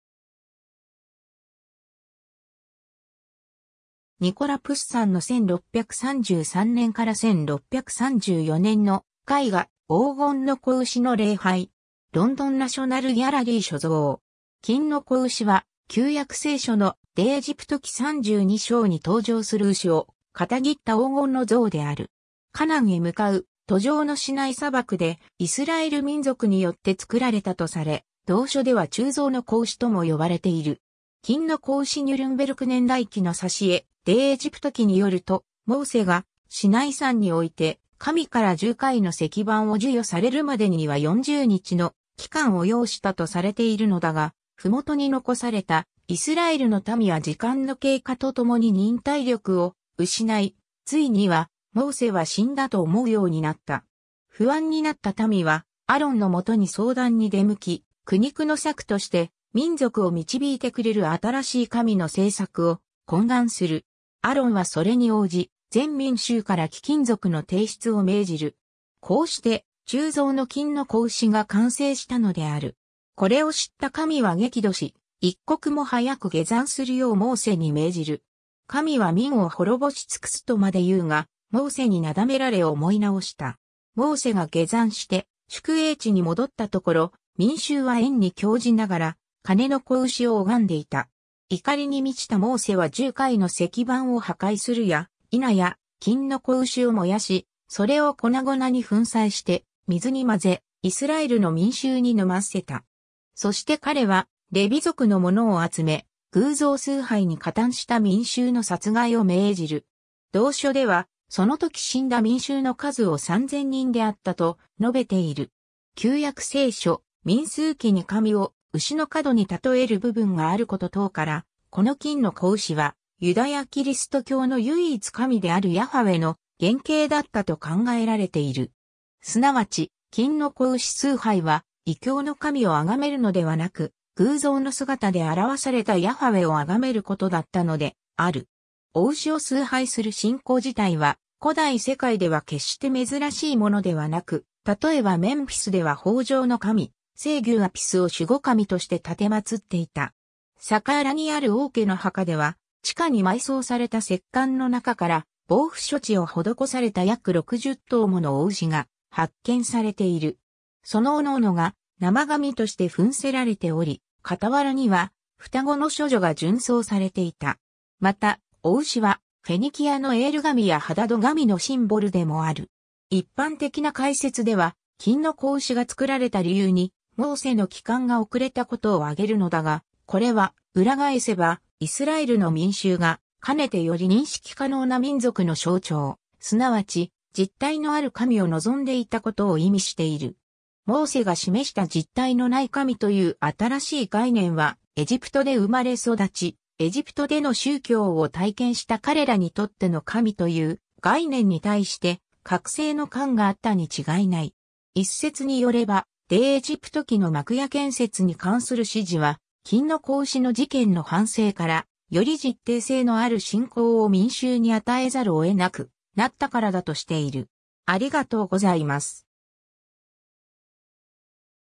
金の子牛
ニコラ・プッサンの1633年から1634年の絵画『黄金の子牛の礼拝』。
ロンドン・ナショナル・ギャラリー所蔵。
 金の子牛（きんのこうし、ヘブライ語: עגל הזהב‎）は、旧約聖書の『出エジプト記』32章に登場する牛を模った黄金の像である。
カナンへ向かう途上のシナイ砂漠でイスラエル民族によって造られたとされ、同書では鋳造の子牛（עגל מסכה）とも呼ばれている。
 金の子牛 ニュルンベルク年代記の挿絵 『出エジプト記』によると、モーセがシナイ山において神から十戒の石版を授与されるまでには40日の期間を要したとされているのだが、麓に残されたイスラエルの民は時間の経過と共に忍耐力を失い、ついには、モーセは死んだと思うようになった。
不安になった民はアロンのもとに相談に出向き、苦肉の策として民族を導いてくれる新しい神の制作を懇願する。
アロンはそれに応じ、全民衆から貴金属の提出を命じる。
こうして鋳造の金の子牛が完成したのである。
 これを知った神は激怒し、一刻も早く下山するようモーセに命じる。
神は民を滅ぼし尽くすとまで言うが、モーセになだめられ思い直した。
モーセが下山して宿営地に戻ったところ、民衆は宴に興じながら金の子牛を拝んでいた。
怒りに満ちたモーセは十戒の石版を破壊するやいなや、金の子牛を燃やし、それを粉々に粉砕して水に混ぜ、イスラエルの民衆に飲ませた。
そして彼はレビ族の者を集め、偶像崇拝に加担した民衆の殺害を命じる。
同書では、そのとき死んだ民衆の数を3千人であったと述べている。
 旧約聖書・民数記に神を牛の角に例える部分があること等から、この金の子牛はユダヤ・キリスト教の唯一神であるヤハウェの原型だったと考えられている。
即ち、金の子牛崇拝は異教の神を崇めるのではなく、偶像の姿で表されたヤハウェを崇めることだったのである。
牡牛を崇拝する信仰自体は古代世界では決して珍しいものではなく、例えばメンフィスでは豊穣の神、聖牛アピスを守護神として奉っていた。
サカーラにある王家の墓では、地下に埋葬された石棺の中から防腐処置を施された約60頭もの雄牛が発見されている。
その各々が生神として扮せられており、傍らには双子の処女が殉葬されていた。
また、牡牛はフェニキアのエール神やハダド神のシンボルでもある。
 一般的な解説では、金の子牛が作られた理由にモーセの帰還が遅れたことを挙げるのだが、これは裏返せば、イスラエルの民衆がかねてより認識可能な民族の象徴、すなわち実体のある神を望んでいたことを意味している。
モーセが示した実体のない神という新しい概念は、エジプトで生まれ育ち、エジプトでの宗教を体験した彼らにとっての神という概念に対して隔世の感があったに違いない。
一説によれば、『出エジプト記』の幕屋建設に関する指示は、金の子牛の事件の反省から、より実体性のある信仰を民衆に与えざるを得なくなったからだとしている。